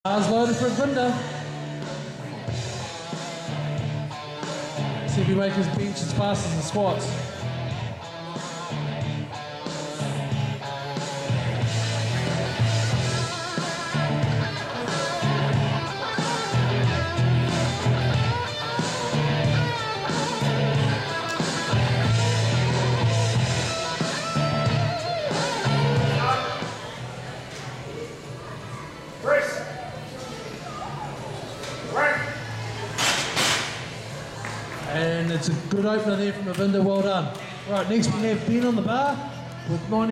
Arvinder
Arvinder Singh, 85kg bench press
Palmerston North, 18 July 2015